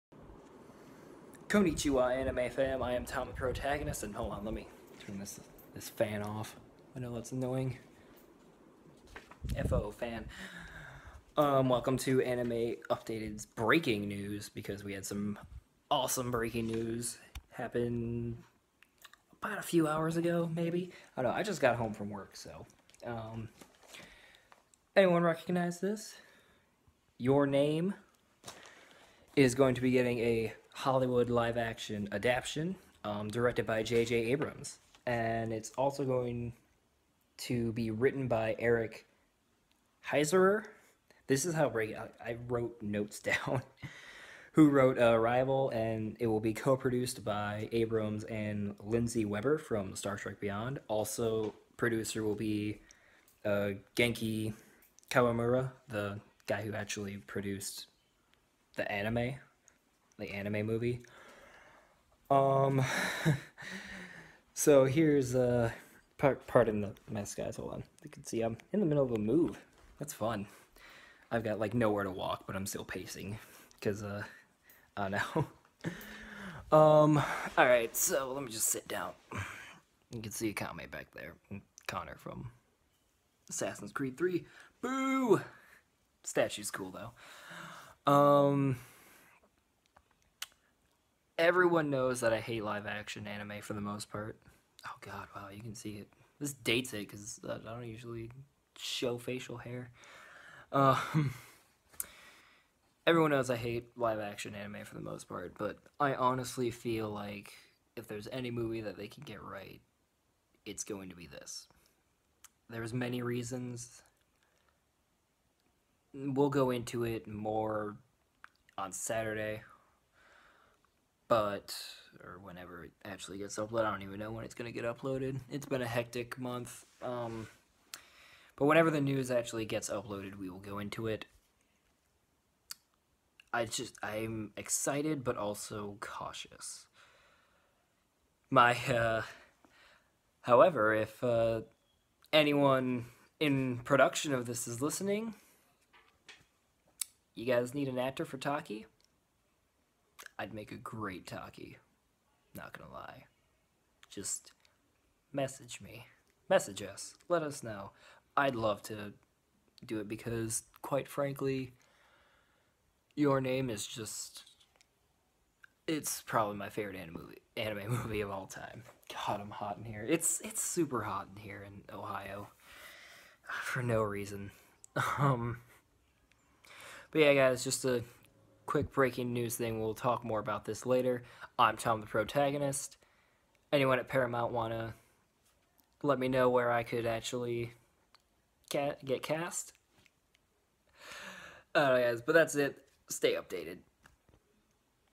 J.J. Abrams to Direct Your Name Live Action!!!! AU Breaking News
It's all in the title... also Please cast me as Taki.
Recorded on my phone... sorry for the bad quality.